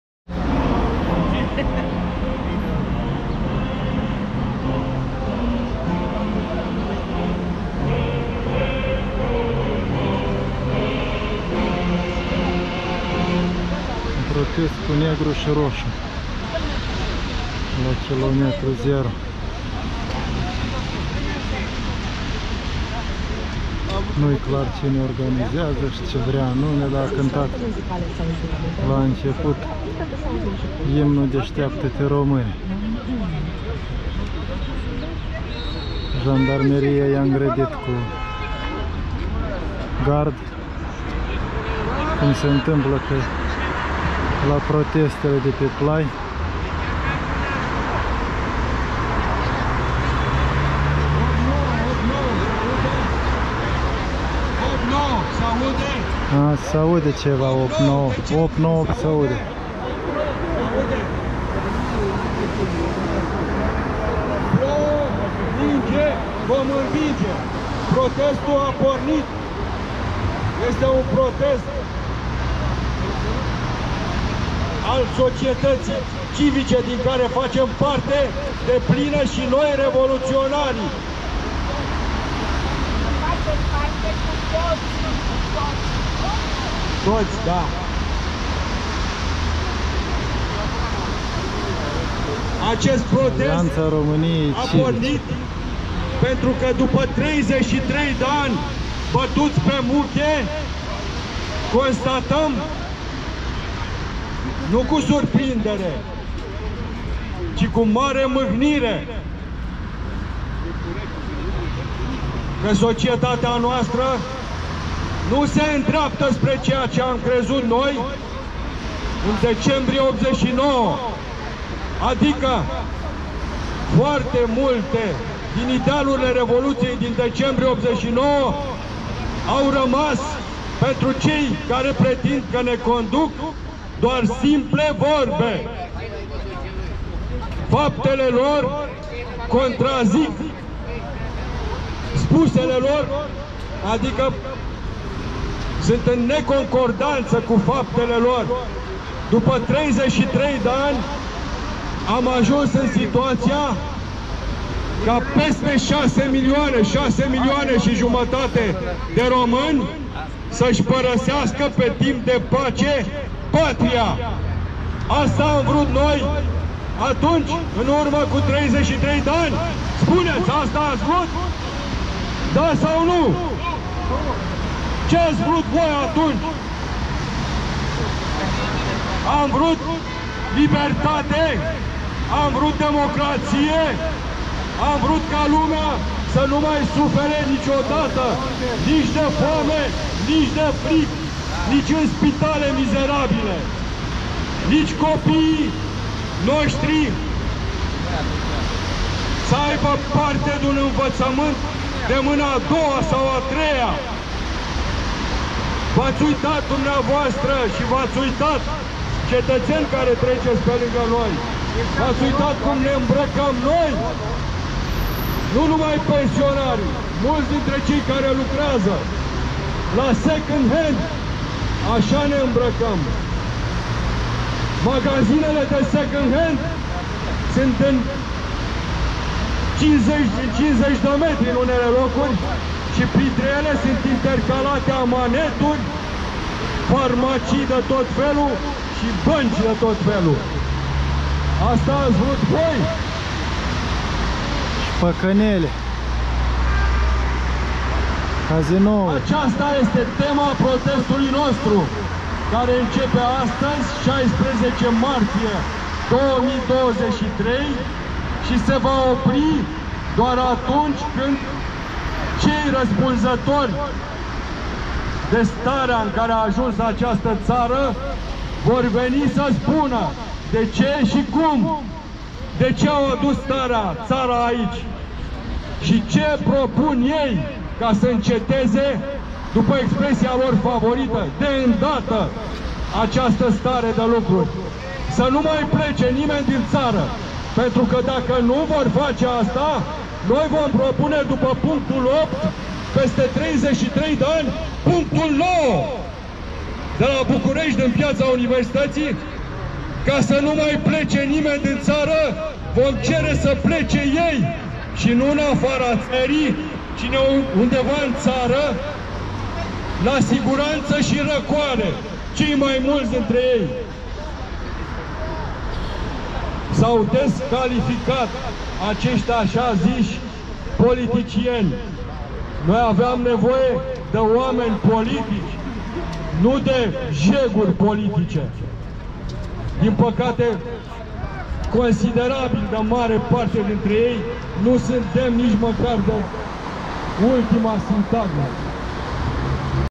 Protest al foștilor revoluționari în Piața Universității - Curaj.TV
Protest supărat, dar cuminte, în țarcul făcut de jandarmii cu pistoale